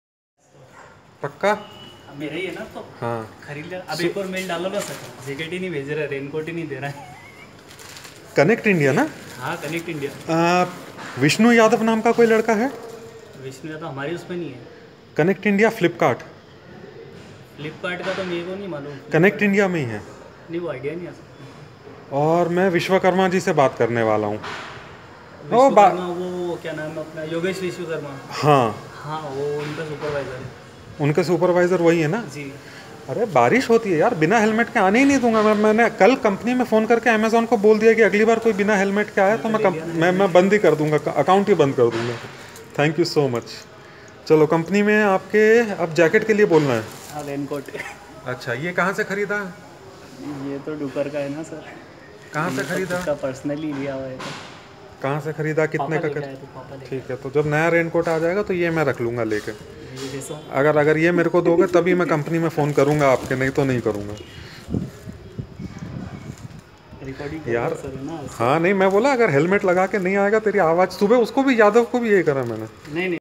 2017 July 28 at 1701hours sweet boy Mubin finally got helmet from ATS ConnectIndia
2017_July_28_at_1701hours_sweet_boy_Mubin_finally_got_helmet_from_ATS_ConnectIndia.mp4